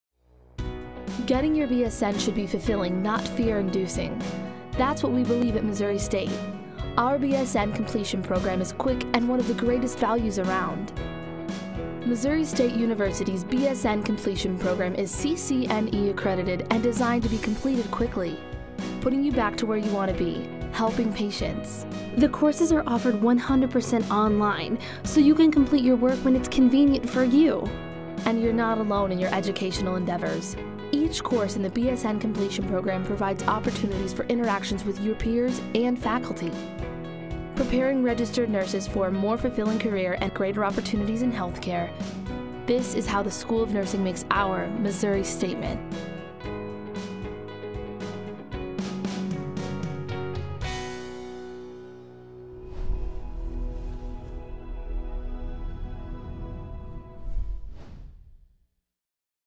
Have a fulfilling career in health care
Engaging. Valuable. Convenient.

Missouri State’s BSN-C program is for registered nurses who are looking to pursue greater career opportunities in health care. The program is CCNE-accredited, courses are 100 percent online and you’ll have support from helpful faculty along the way.